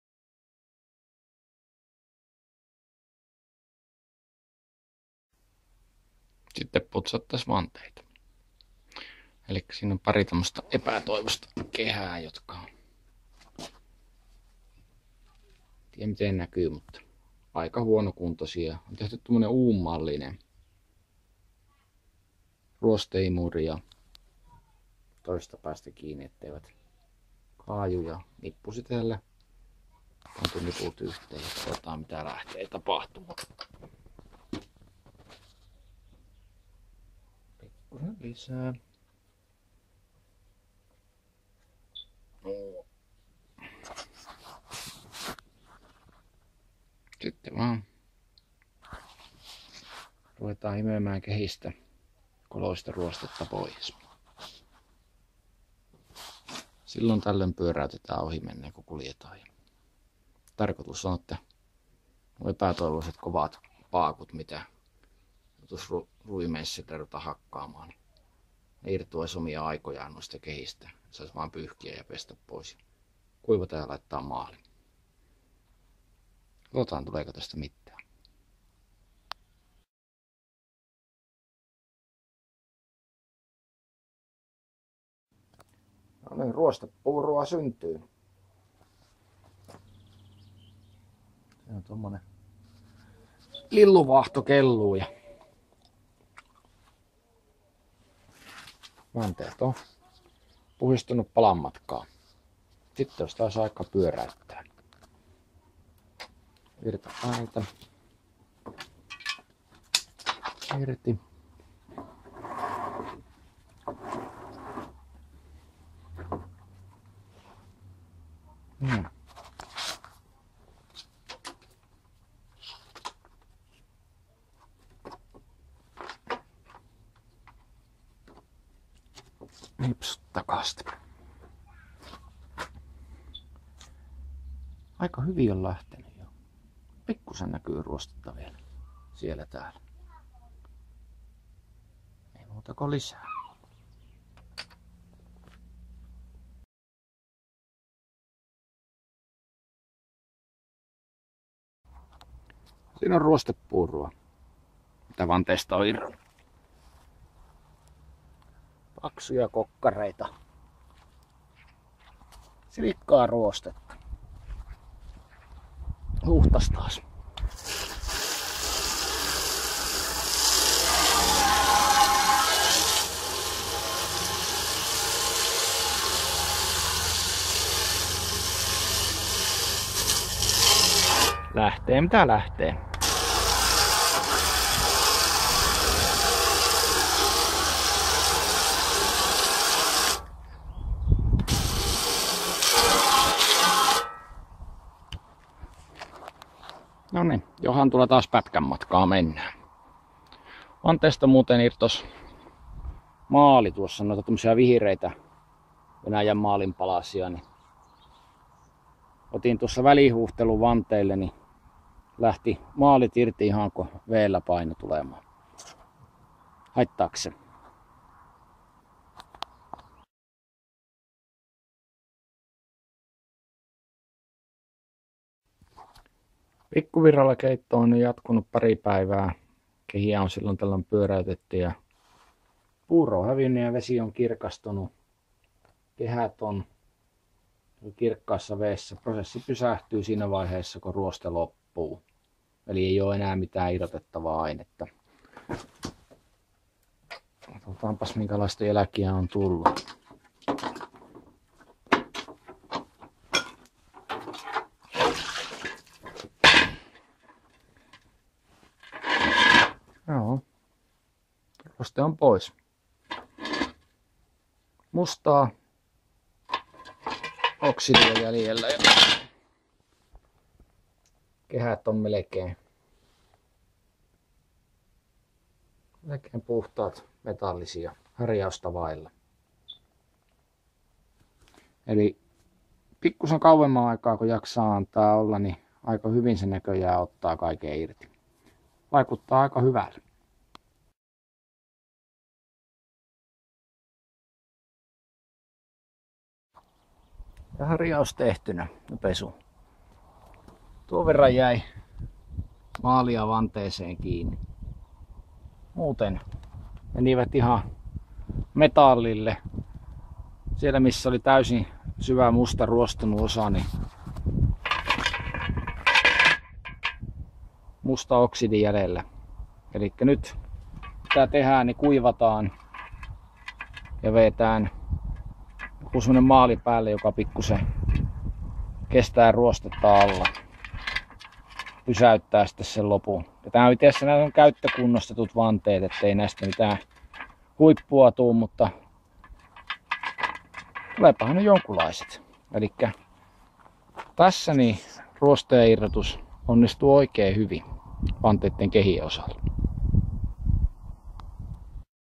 Vannekehien putsaus
Ruostetta irti sähköllä